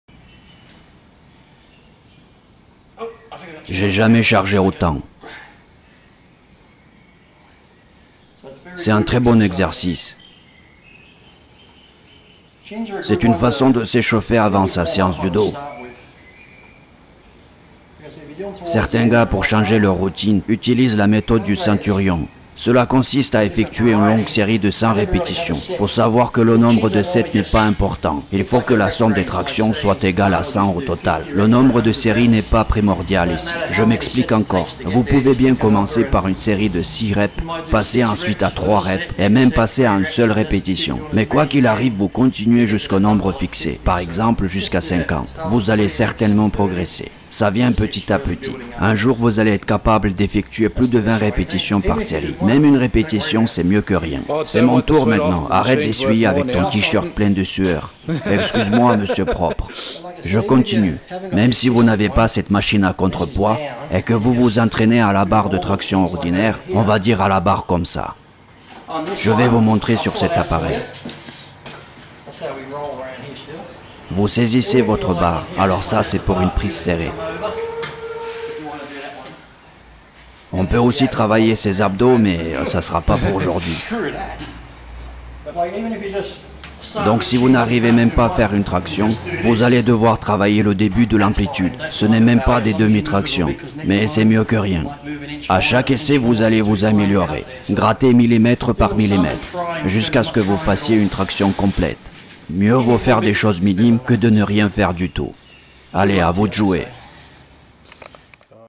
Lee Priest et la méthode du centurion FR
Lee Priest parle de la méthode du Centurion et motive les nuls en tractions

Le contenu de la vidéo est tiré de la chaîne youtube: Sam's Fitness - Gym Equipment